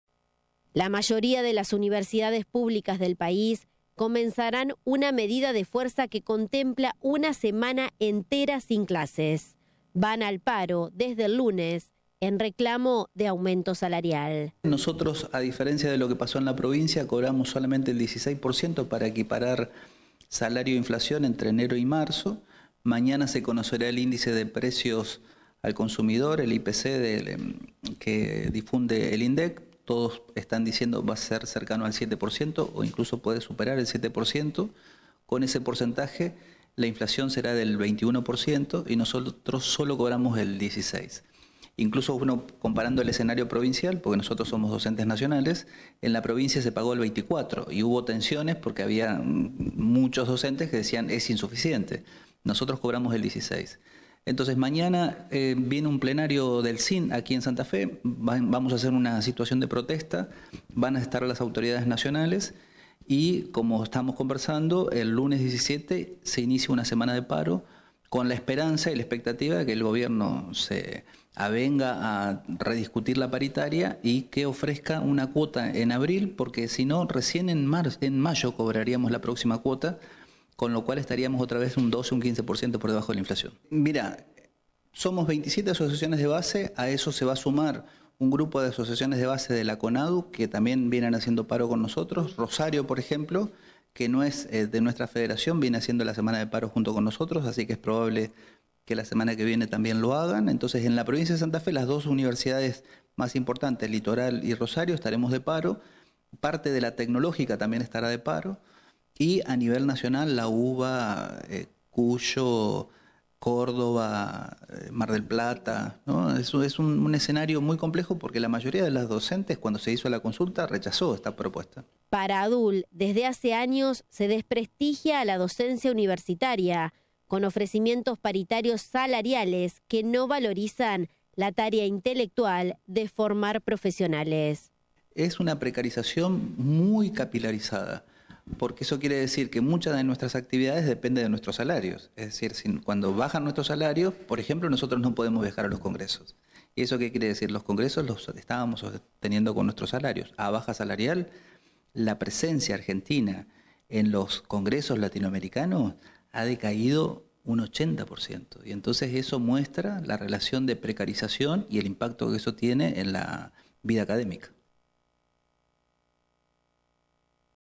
Paro de docentes universitarios la próxima semana
Oscar Vallejos, titular de Adul, explicó los motivos de la medida de fuerza.